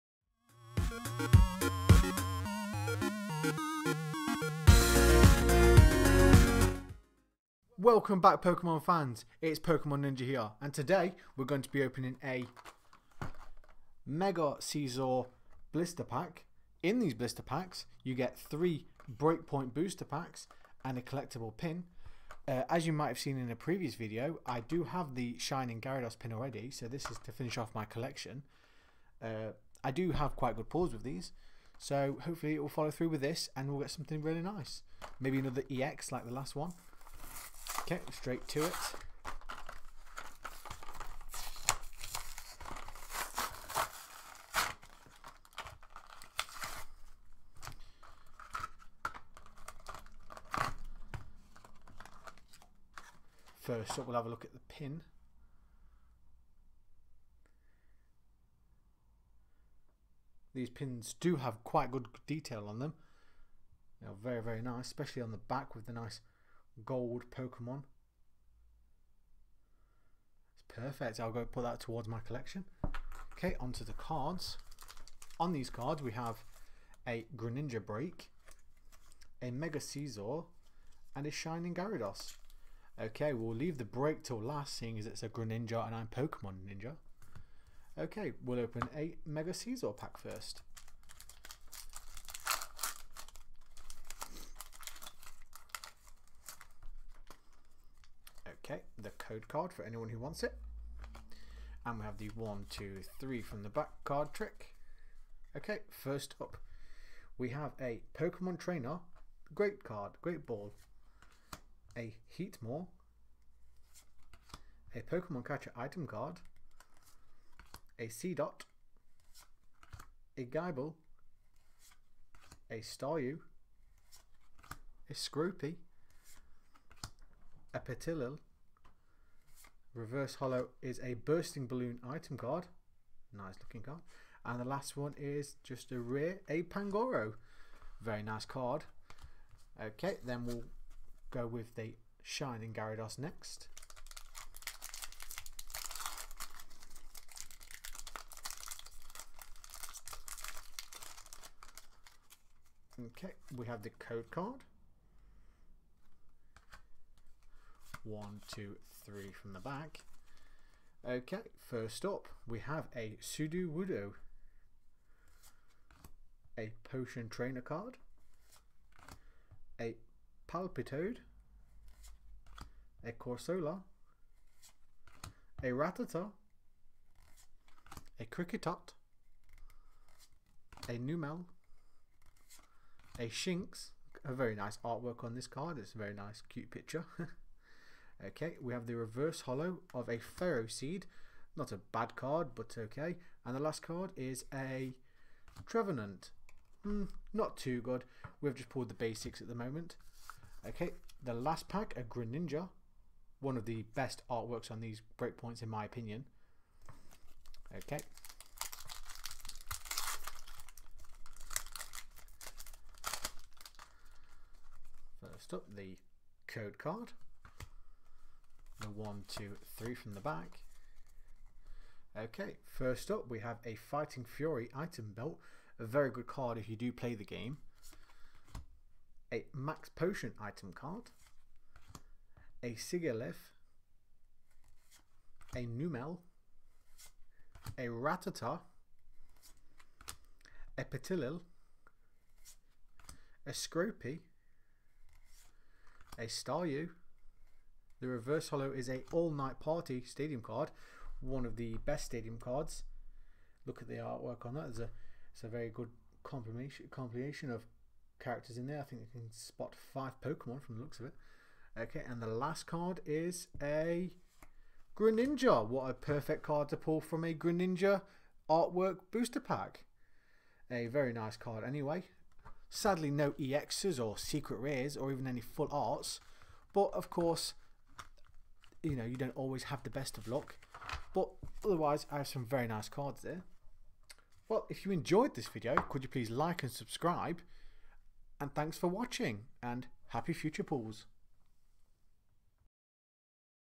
Pokemon BREAKpoint M Scizor Blister Pack Opening!
Hi Today i will be opening a BREAKpoint Scizor Blister pack!

Do i get anything good? Watch and See :P